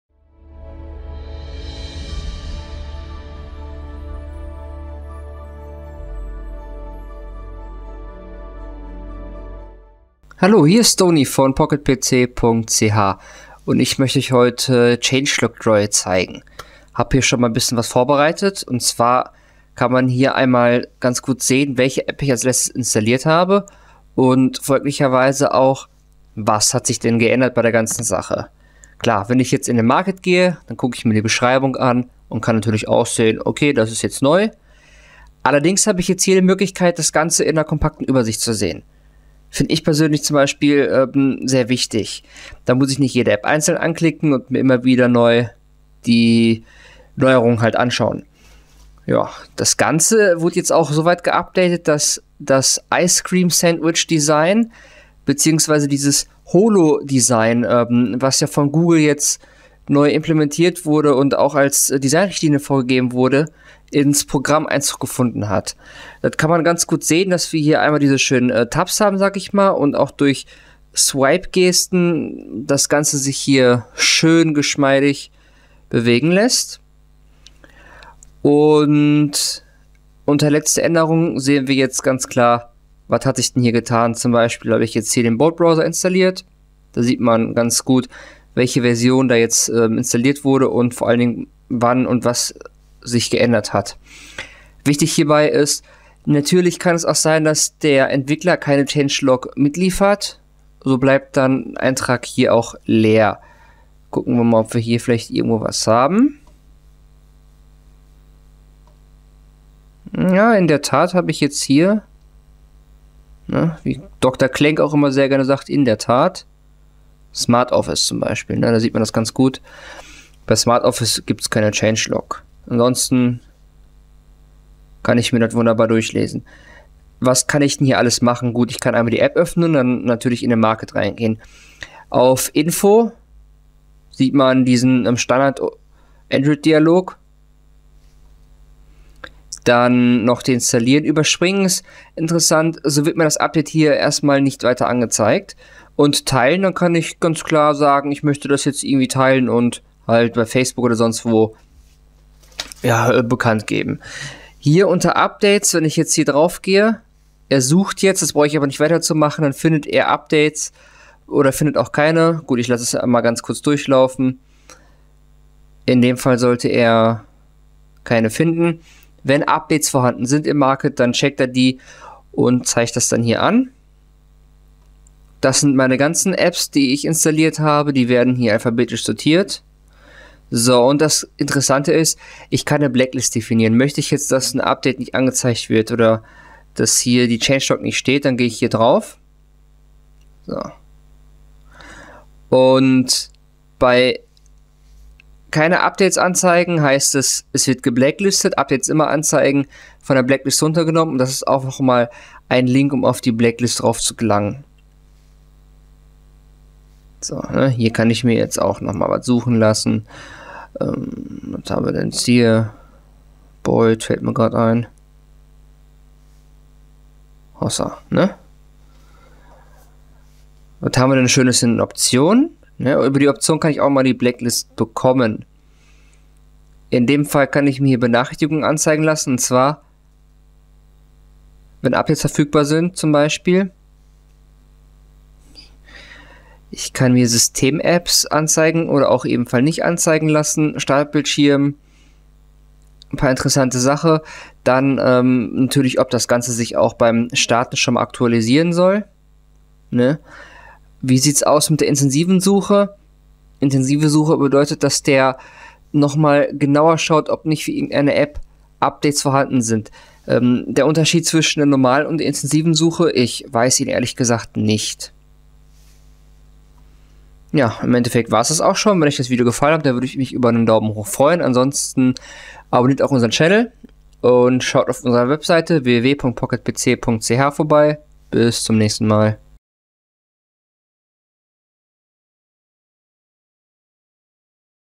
Changelog Droid für Android vorgestellt
zeigt euch hier die App Changelog Droid. Diese Anwendung listet einem alle Änderungen an den zuletzt aktualisierten Apps und Updates an einem Ort auf. Damit ist es nicht mehr nötig, bei jeder einzelnen App, für die ein Update bereit steht, die Neuerungen herauszusuchen.